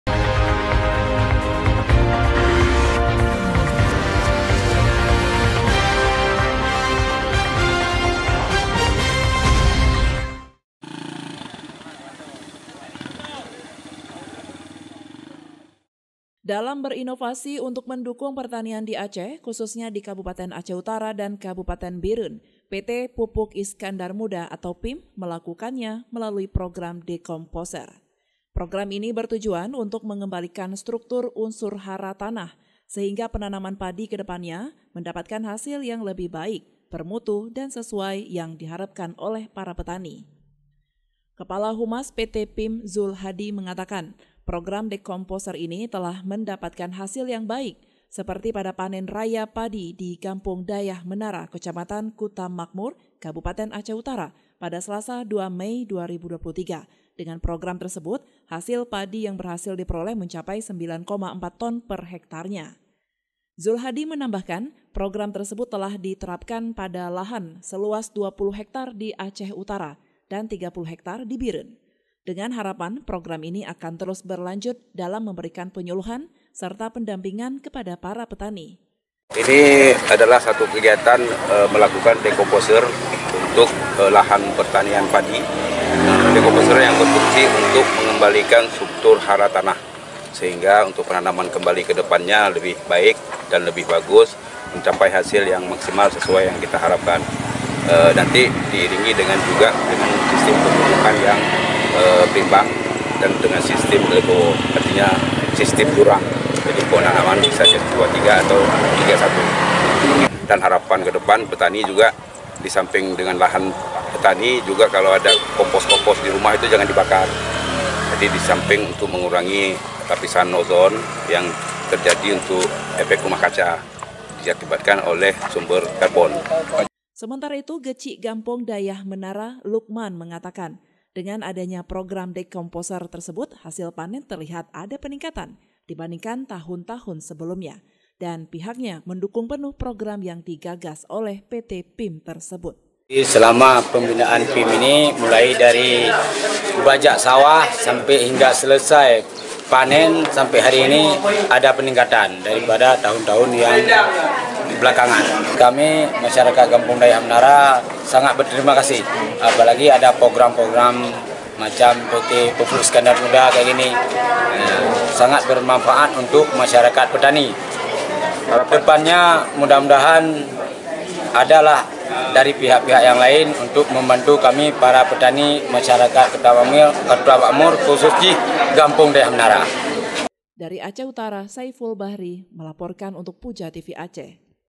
Program Dekomposer PT PIM Sukses Dongkrak Panen Padi Di Aceh Utara
Dengan memanfaatkan bahan organik kompos jerami atau dekomposer yang di gagas oleh  PT PIM bersama masyarakat tani di Aceh Utara, mampu mendongkrak hasil panen gabah dari 6,5 ton per hektar menjadi 9,6 ton per hektar.